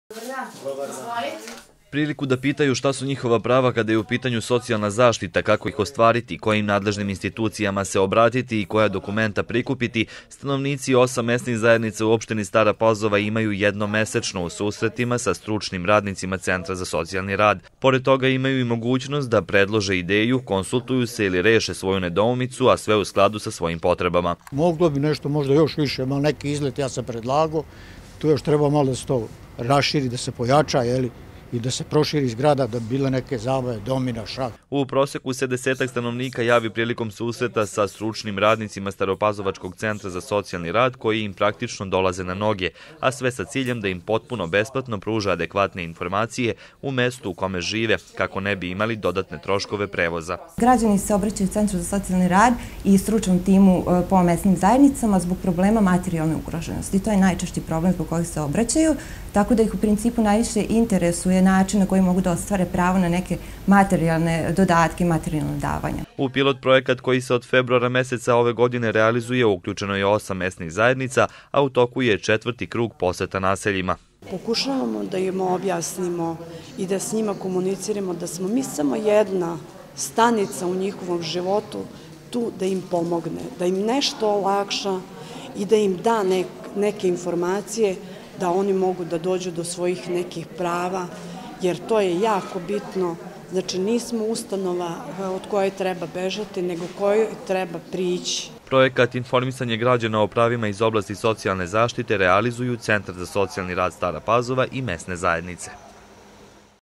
Informisanje građjana na terenu o pravima socijalne zaštite
Sa ciljem da se stanovnici opštine Stara Pazova što bolje upoznaju sa pravima u oblasti socijalne zaštite i načinu ostvarivanja istih, staropazovački Centar za socijalni rad u saradnji sa Mesnim zajednicima sprovodi pilor projekat „Inforimisanje građana o pravima iz oblasti socijalne zaštite“. U projekat su uključeni stanovnici 8 naselja u opštini.